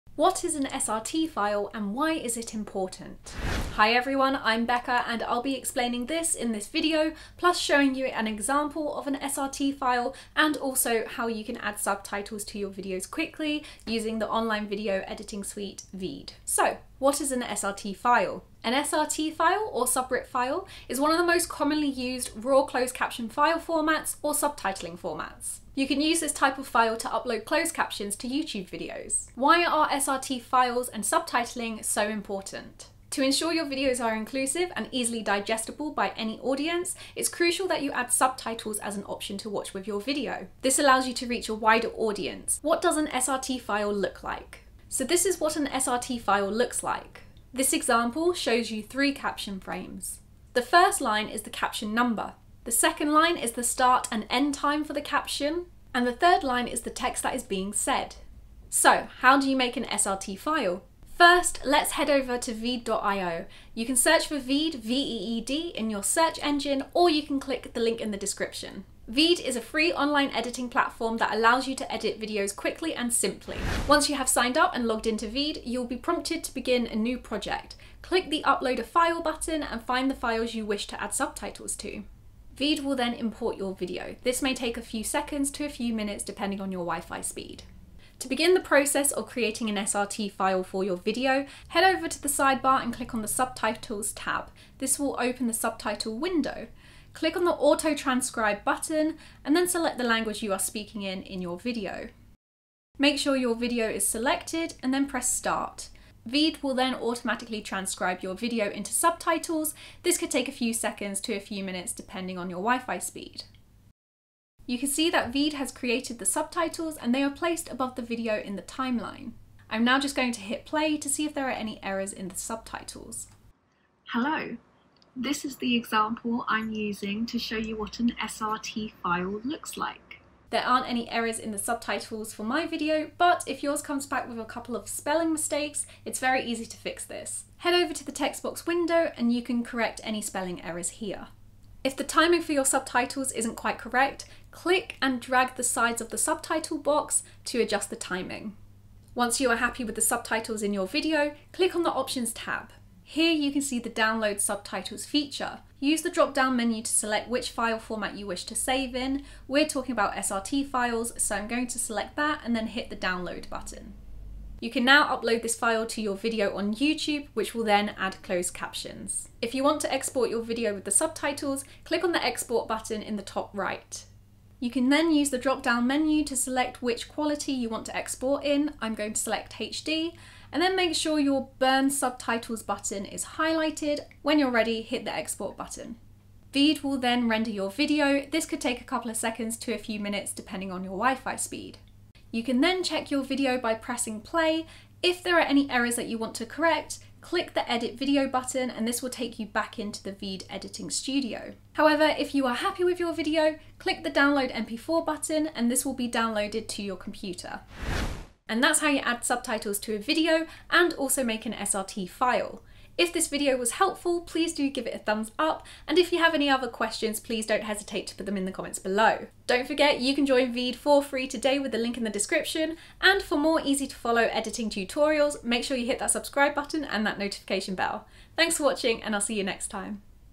What is an SRT File and How do I Make One?
Learn what an SRT file is and how to make one!

In this short video, we teach you what an SRT File is, how to add subtitles to a video and how to make an SRT File, all using Veed, the online editing software.

What is an SRT File and How to Make One:

An SRT file or SubRip file is one of the most common types of raw closed caption file formats or subtitle formats. You can use this type of file to upload closed captions to a YouTube video.

To ensure your videos are inclusive and easily digestible for any audience, it's crucial you include an option to watch with subtitles. This will allow you to reach a wider audience too. Also more and more people tend to watch videos without any sound, making subtitles even more important in helping your audience be engaged in your videos.

What does an SRT File look like?

The example on screen shows you three caption frames.

The first line is the caption number.
The second line is the start and end time for the caption.
And the third line is the text that is being said.

How do you Make an SRT File?

First head over to Veed.io. Veed is a free online video editing platform that allows you to edit videos quickly and easily.

Once you have signed up and logged in, Veed will prompt you to upload footage. Press ‘New Project’, then click ‘Upload a File’ and find the files you wish to use.

Click on the ‘subtitles’ tab on the sidebar, and select ‘Auto Transcribe’. Veed will then automatically convert your audio to subtitles.

Once Veed has automatically generated your subtitles, you can see them located on top of your video file, in the timeline.

Press play and check if there are any spelling errors in the subtitles. If there are errors, head to the text box window, and correct them.

Once the text is correct, head to the ‘Options’ tab, and you will be given the option to ‘Download Subtitles’.

To download the SRT file, make sure the drop down menu has selected .SRT. Then hit the ‘download’ button. This file will then be downloaded and saved to your computer.

You can then upload the SRT file to your YouTube video.

If you want to export the video with the subtitles, head over to the ‘Export’ button, and make sure the ‘Burn Subtitles’ button is highlighted. Using the dropdown menu, select what resolution you want to export in. Then hit ‘Export Video’.

Veed will then render your video.

Once Veed has finished, you can check the video one last time, by hitting ‘Play’. If you want to reedit the video, hit the ‘Edit Video’ button, and this will take you back to the editing page. If you are happy with the video, hit ‘Download MP4’.

How to Make an SRT File Step by Step:

00:00 Intro
00:16 What is an SRT File?
00:29 Why are Subtitles and SRT Files Important?
00:42 What does an SRT File Look Like?
00:59 Join Veed
01:31 How to Add Subtitles to a Video
02:29 How to Save an SRT File
02:49 How to Export a Video with Subtitles
03:28 Outro

Good luck!

- 4k video quality
- Longer videos
- Custom fonts

Becca & VEED.IO